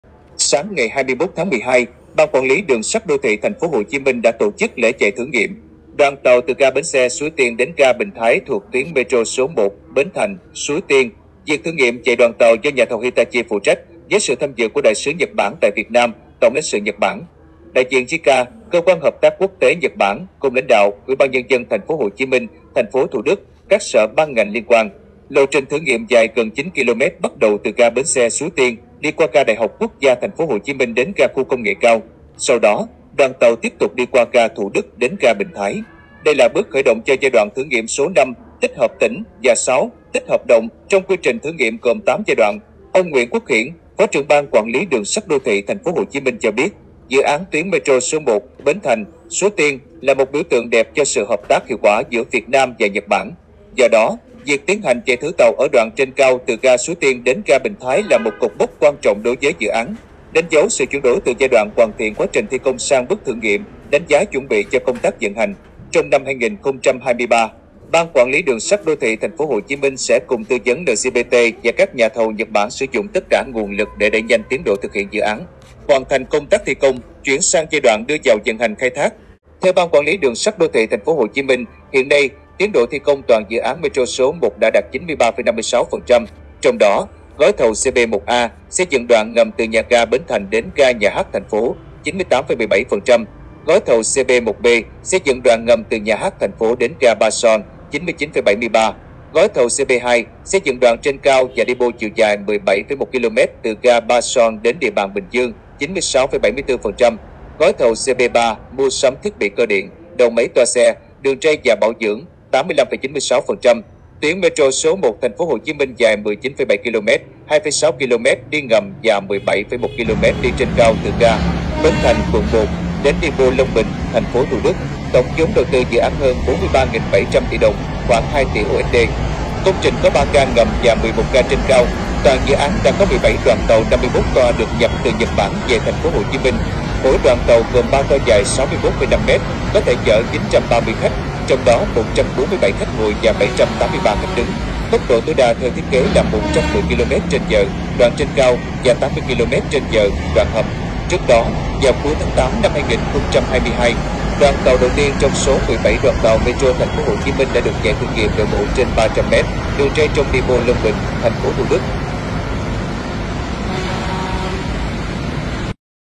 Chạy thử nghiệm tàu Metro tuyến Bến Thành - Suối Tiên
10h sáng nay (21/12/2022), đoàn tàu metro số 1 (Bến Thành - Suối Tiên) chính thức lăn bánh chạy thử nghiệm đoạn dài gần 9km, bắt đầu từ ga bến xe Suối Tiên đến ga Bình Thái (TP Thủ Đức)...

Tuyến metro số 1 TPHCM dài 19,7 km (2,6 km đi ngầm và 17,1 km đi trên cao) từ ga Bến Thành (quận 1) đến depot Long Bình (TP Thủ Đức). Tổng vốn đầu tư dự án hơn 43.700 tỷ đồng (khoảng 2 tỷ USD). Công trình có 3 ga ngầm và 11 ga trên cao. Toàn dự án đã có 17 đoàn tàu 51 toa được nhập từ Nhật Bản về TPHCM.

Mỗi đoàn tàu gồm ba toa dài 61,5 m, có thể chở 930 khách. Tốc độ tối đa theo thiết kế là 110 km/h (đoạn trên cao) và 80 km/h (đoạn đi ngầm).
THAM KHẢO THÊM CÁC DỰ ÁN:
➤ Thông tin các dự án Khu đô thị Nam Long Group phát triển